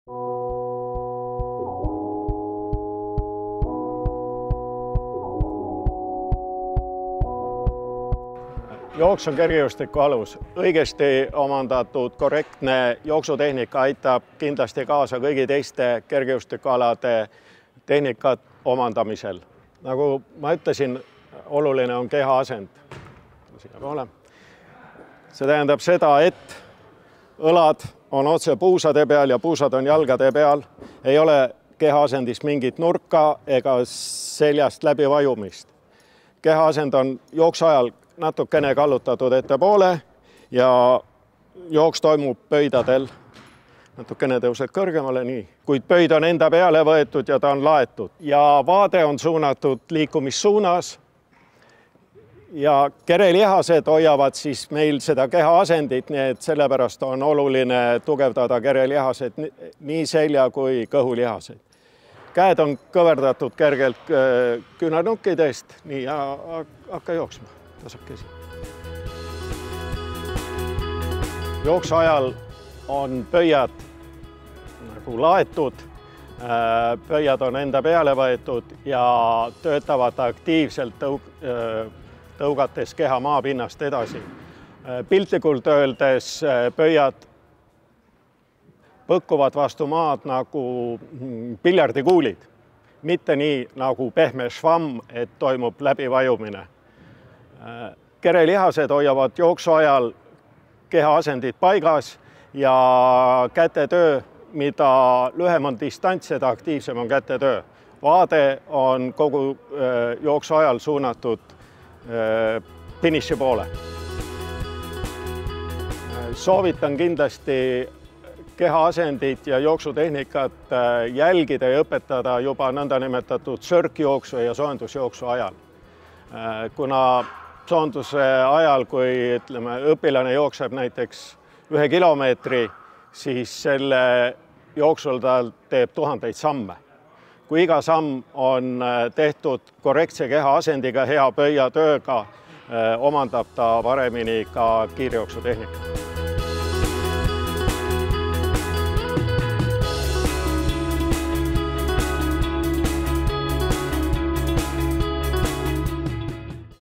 EKSL interaktiivne õppeprogramm, kergejõustik I osa
Eesti Koolispordi Liidu interaktiivne õppeprogramm. Kergejõustiku harjutusi alustame kiirjooksu tehnikaga. Juhendab teenekas kergejõustiku treener Valter Espe.